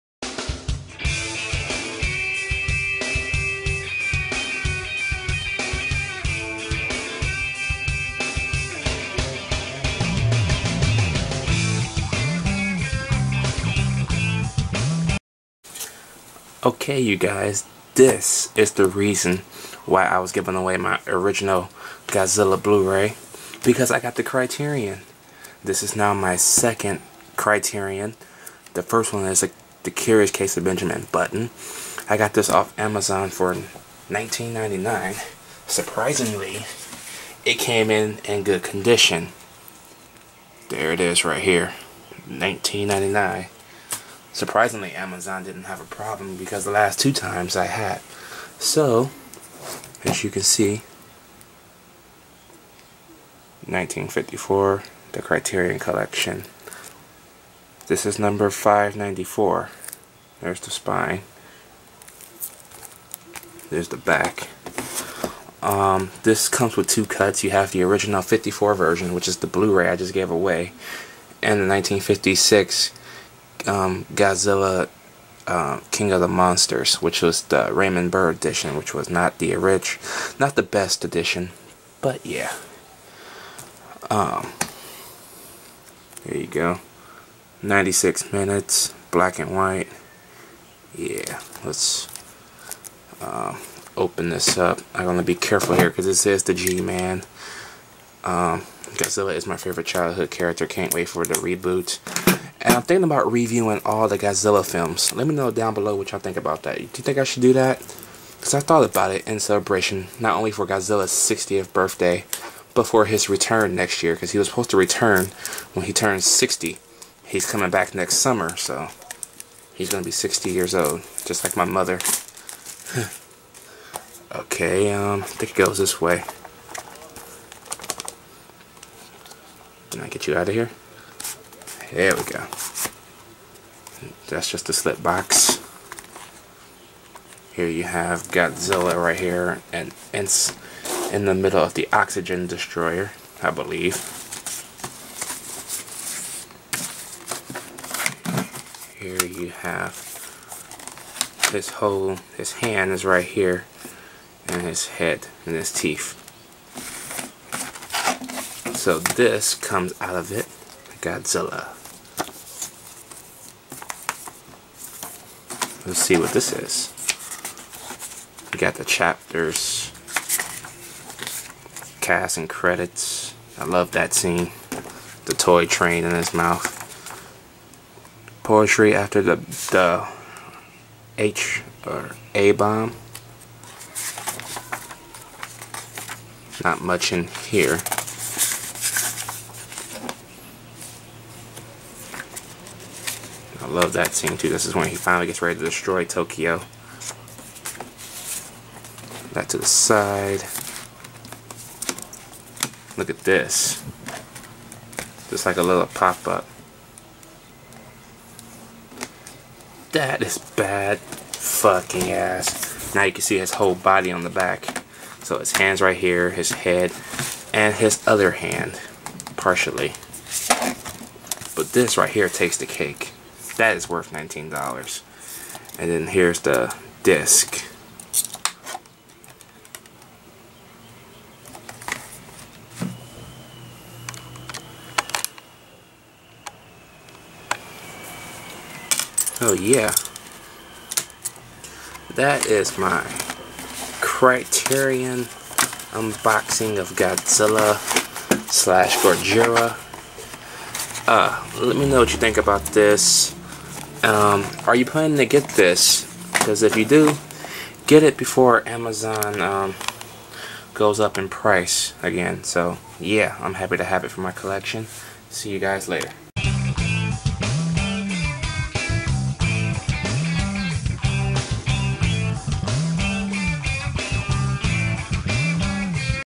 Godzilla Criterion Blu-Ray Unboxing
My Unboxing of the Godzilla Criterion Enjoy the Show!...

Studio: Criterion Collection, Toho
Ruuning Time: 1 Hr. 36 Mins.